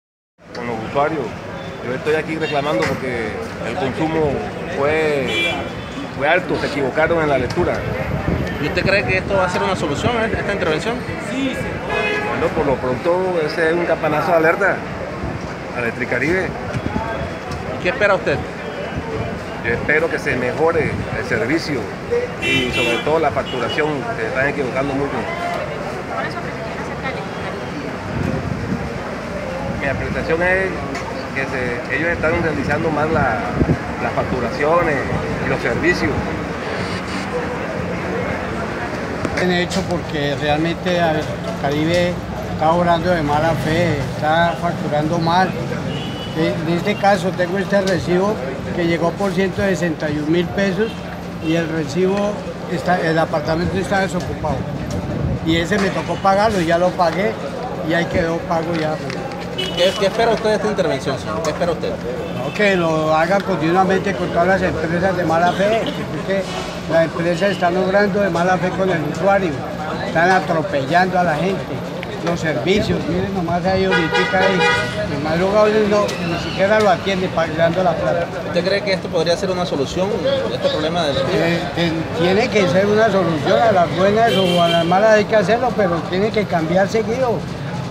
Usuarios de Electricaribe en Santa Marta exigen mejoras
Ante la oficial intervención del Gobierno a Electricaribe, este es el panorama de oficina en Santa Marta.